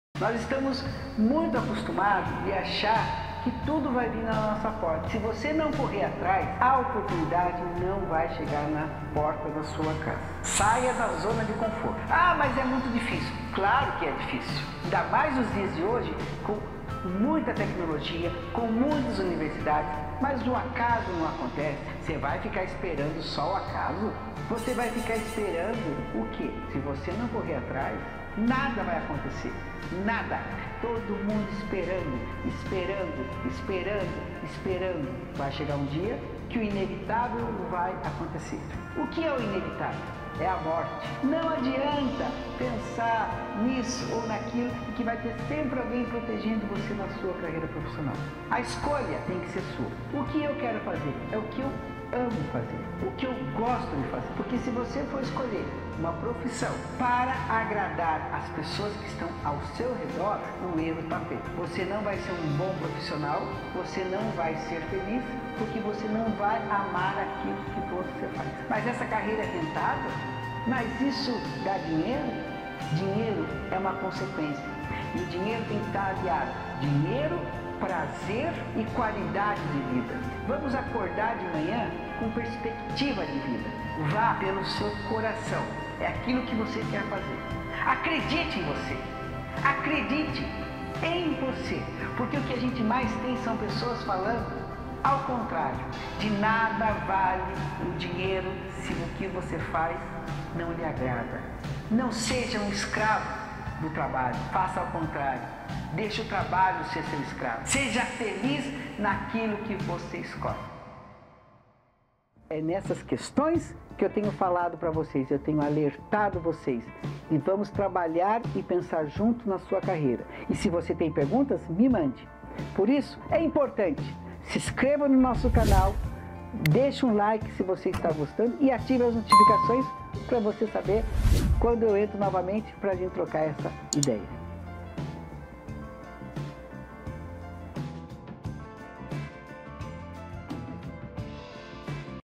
Motivacional ''Acredite em Você!'' -Junior Mosko
Vamos relembrar! Pelo o que você está esperando? Vá pela sua escolha e acredite em você, quando escolher a profissão que você gosta está fazendo o trabalho como seu escravo.